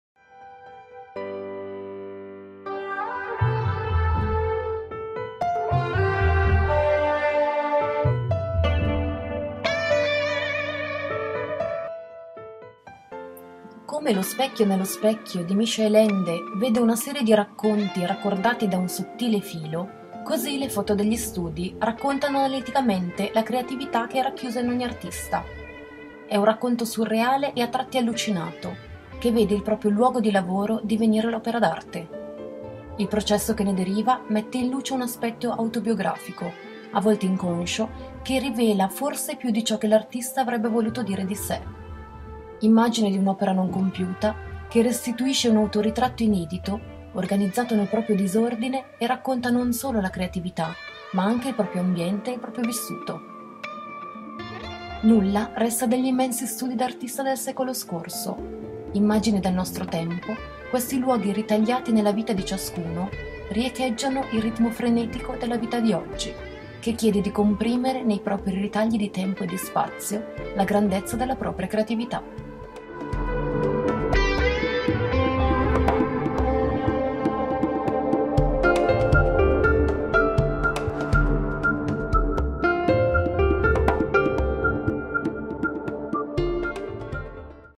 100%creative
100%creative
Cristina Cattaneo, Simone Garbin, Hugo Garza, Giovanni Greco, Claudia Ielmoni, Kevin Jackson, Nicholas Tolosa, Olga Vanoncini

LO SPECCHIO NELLO SPECCHIO
Come lo specchio nello specchio di Michael Ende vede una serie di racconti raccordati da un sottile filo, così le foto degli studi raccontano analiticamente la creatività che è racchiusa in ogni artista.
è un racconto surreale e a tratti allucinato, che vede il proprio luogo di lavoro divenire l'opera d'arte. Il processo che ne deriva mette in luce un aspetto autobiografico, a volte inconscio, che rivela forse più di ciò che l'artista avrebbe voluto dire di sé.
Immagine di un'opera non compiuta, che restituisce un autoritratto inedito, organizzato nel proprio disordine e racconta non solo la creatività, ma anche il proprio ambiente e il proprio vissuto.
Nulla resta degli immensi studi d'artista del secolo scorso: immagini del nostro tempo, questi luoghi ritagliati nella vita di ciascuno, riecheggiano il ritmo frenetico della vita di oggi, che chiede di comprimere nei propri ritagli di tempo e di spazio la grandezza della propria creatività.
Curatela | Anna Epis